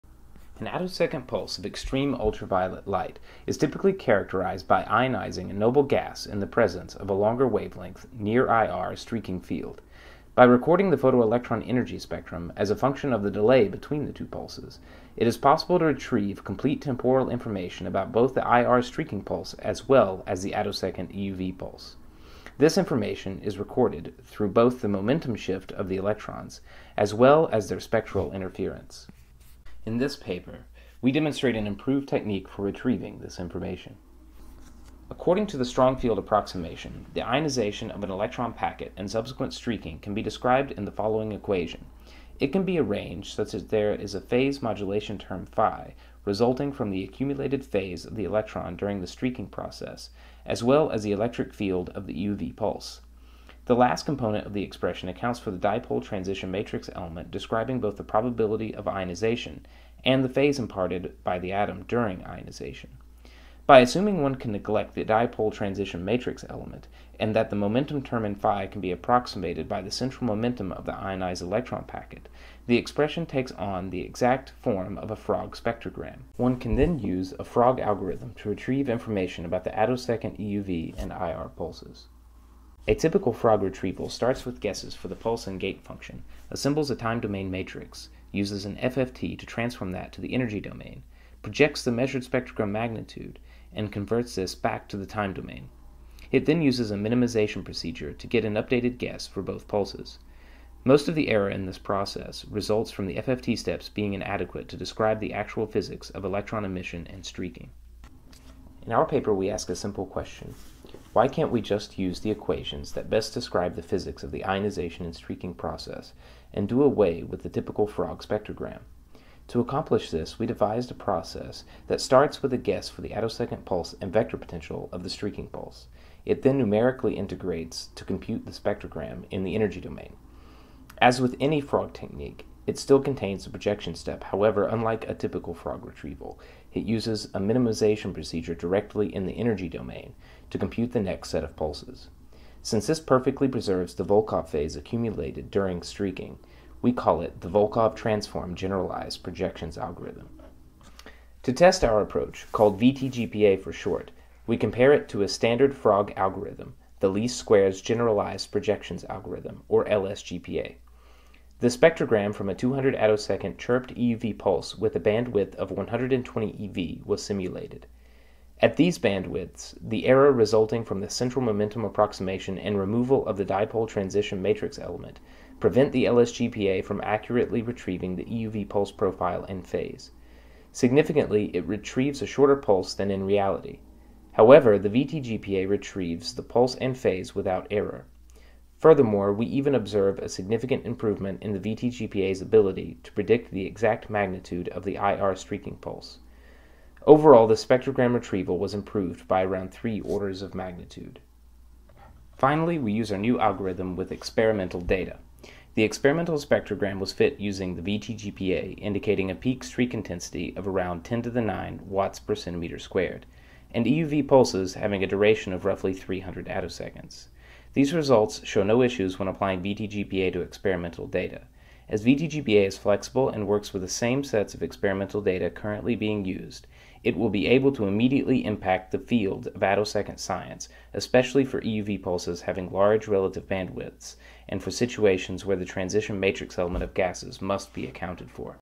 Volkov transform generalized projection algorithm for attosecond pulse characterization
Video abstract for the article ‘Volkov transform generalized projection algorithm for attosecond pulse characterization‘ by P D Keathley, S Bhardwaj, J Moses, G Laurent and F X Kärtner (P D Keathley et al 2016 New J. Phys. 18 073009).